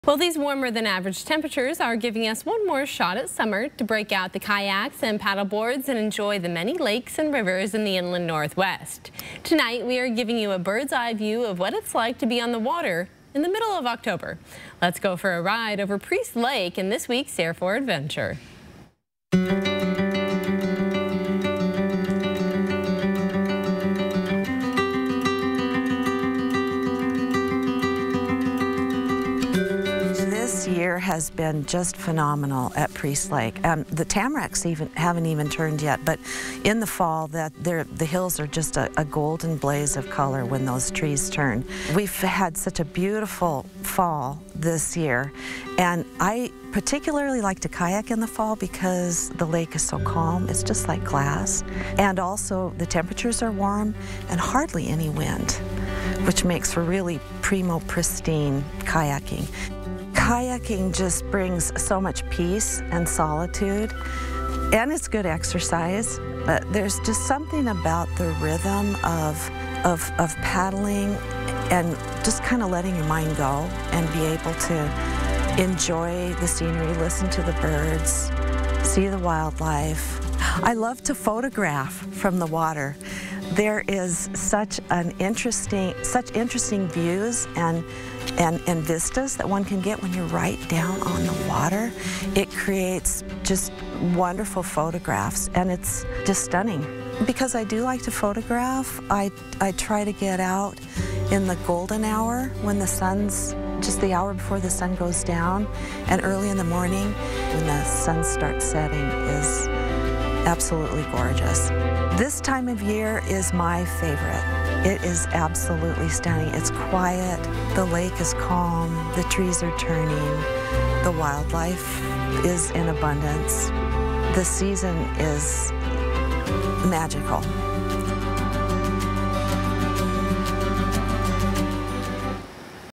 Air 4 Adventure: Kayak at Priest Lake before time runs out
PRIEST LAKE, ID. — These last few days have seen warmer-than-average temperatures. This gives us a few more opportunities to enjoy the lakes and rivers we have in the Inland Northwest!

With that in mind, we want to give you a bird’s eye view of what it’s like to be on the water in the middle of October.

Let’s go for a ride over Priest Lake in this week’s Air 4 Adventure!